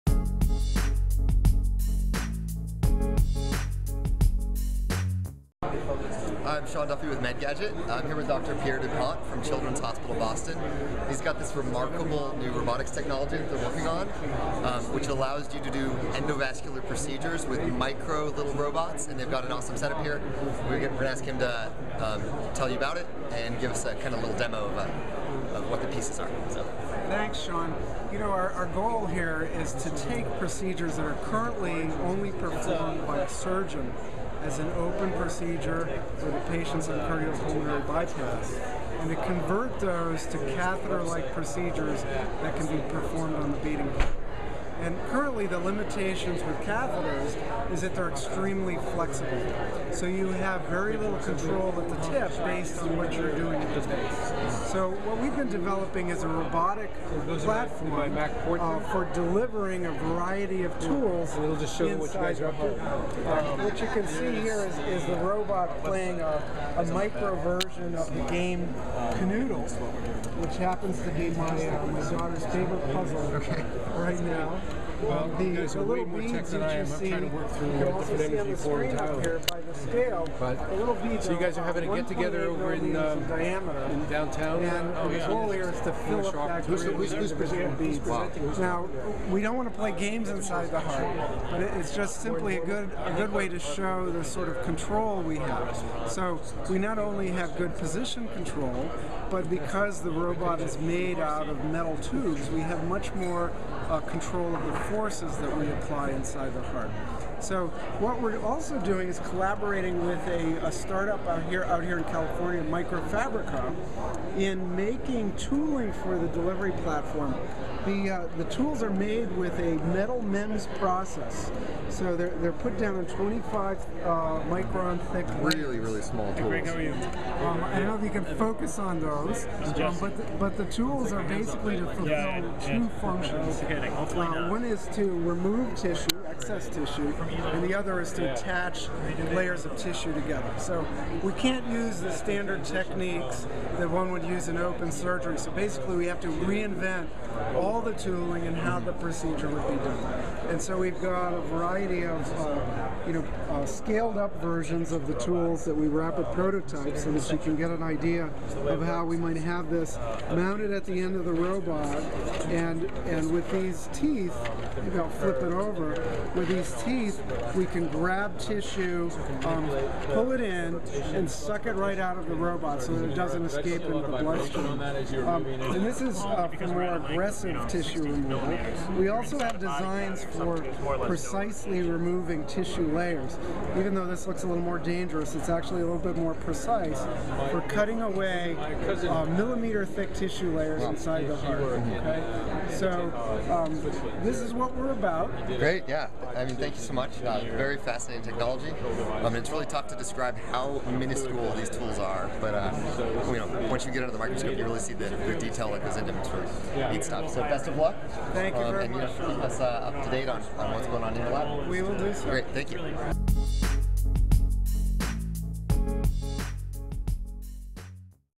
Microrobotic Vascular Surgery System at TEDMED2010
Here's a catheter-based microrobotic vascular surgery system from Boston Children's Hospital that was presented at TEDMED 2010.